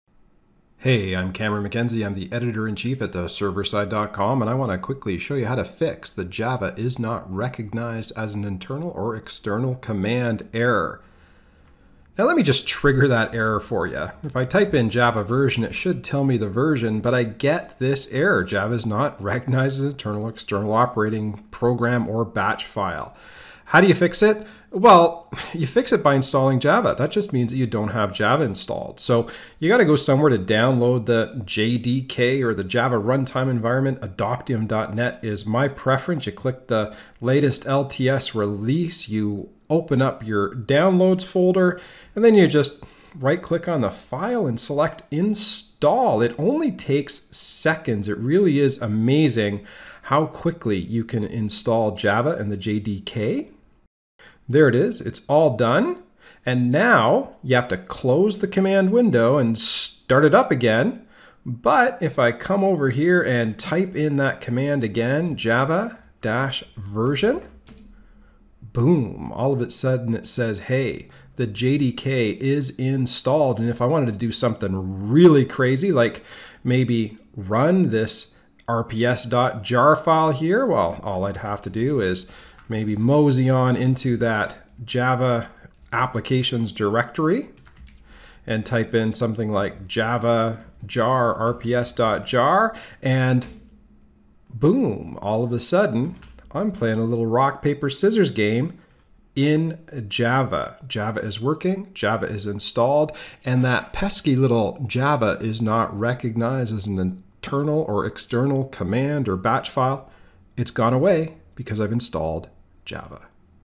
Fix 'Java Not Recognized as an Internal or External Command, Operable Program or Batch' Error
Have you run into the 'Java not recognized' error? Is your computer telling you that Java is not an internal or external command? If that's the problem you've got, don't worry, there's an easy fix. Just install Java or the JDK, make sure you have added Java's bin directory to the PATH, and you have restarted your command prompt recently. That'll fix it!